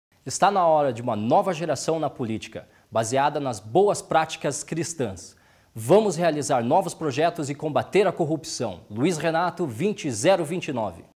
Luiz Renato 20029 - PSC
Propaganda Eleitoral Gratuita - Lei Eleitoral 9504/97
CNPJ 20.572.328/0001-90
Coligação Todos Pelo Paraná
PSDB - DEM - PSB - PSD - PTB - PP - PPS - PSC - PR - PROS - SD - PSL - PSDC - PMN - PHS - PEN - PT do B